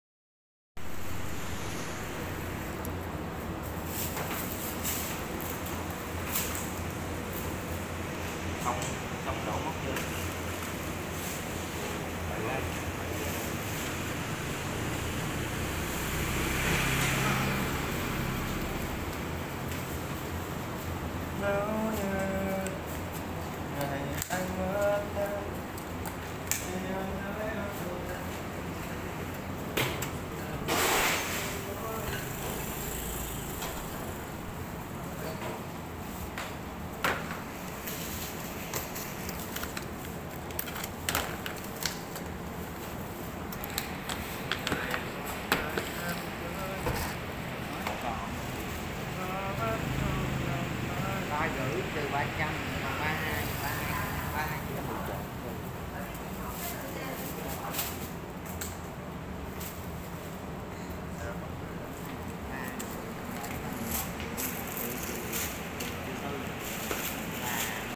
TP HCM, Sài Gòn mì trộn tên lửa hấp dẫn quá,quay phim YouTube _ văn quý vlog cuộc sống Việt
đường 8a khu tên lửa phường Bình Trị Đông b quận Bình Tân TP HCM